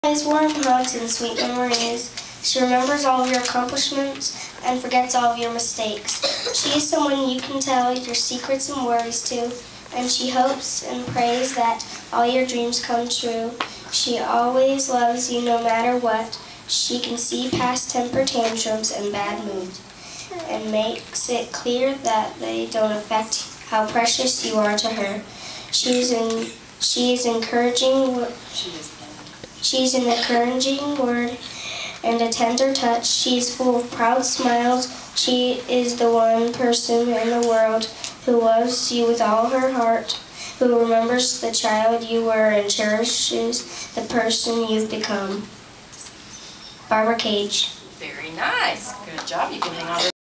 Kimmie Grandma.AVI
Kimmie reads the poem "What Is A Grandma?" by Barbara Cage during the practice for Grandparents Day 2010 Crosby Park Elementary!